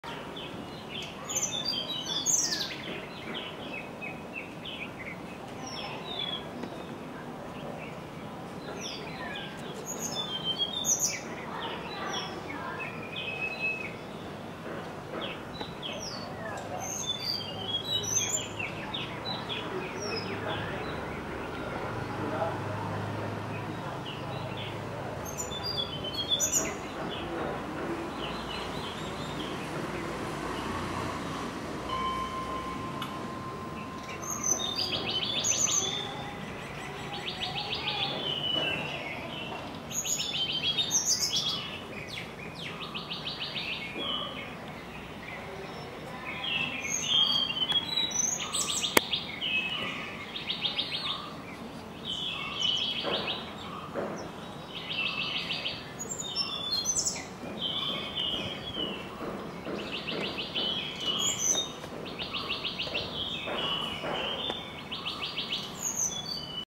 Tickell's Blue Flycatcher (Cyornis tickelliae) | टिकेलचा निळा माशीमार
Tickell's Blue Flycatcher (Cyornis tickelliae) is a small, striking bird with vibrant blue plumage. The male has a bright blue back, wings, and tail, with a white underside and a pale orange throat. Its forehead and eye rings are white, and it has a thin black stripe above its eyes. The female is a duller grey-blue, with a white underside and a pale yellow throat. Both sexes have a slender build and a long, black tail. They are known for their swift, agile flight and their habit of catching insects in mid-air. Their call is a soft, melodic whistle, often described as a series of descending notes.